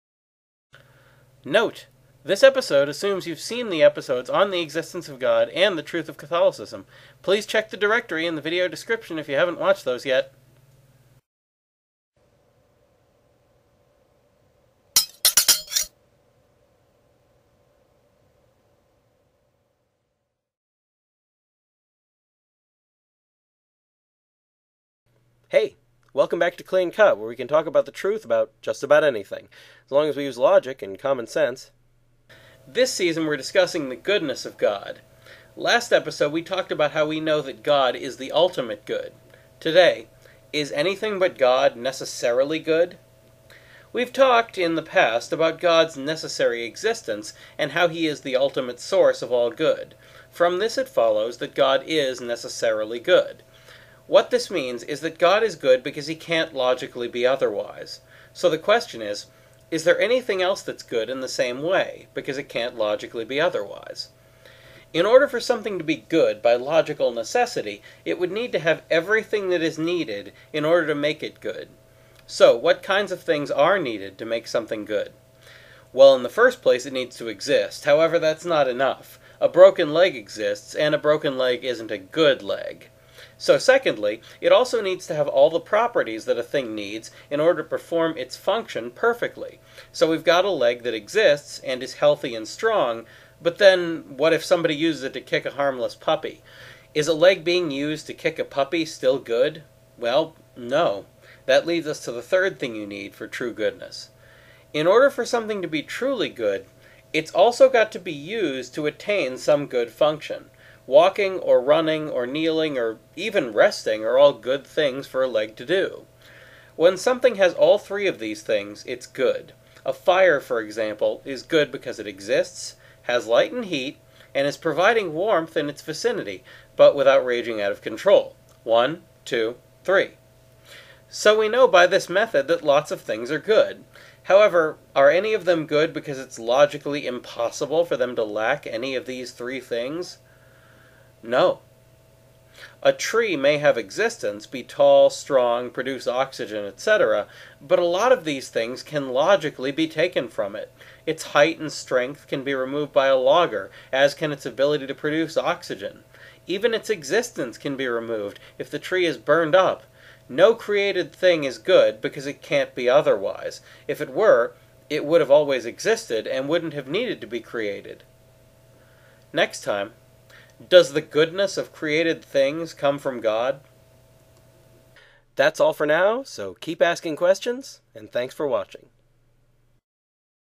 Is Anything But God Necessarily Good?
What is required, in order for something to be necessarily-good?  Can the requirements for goodness be taken away from created things?  Can any created thing have the requirements for goodness by necessity?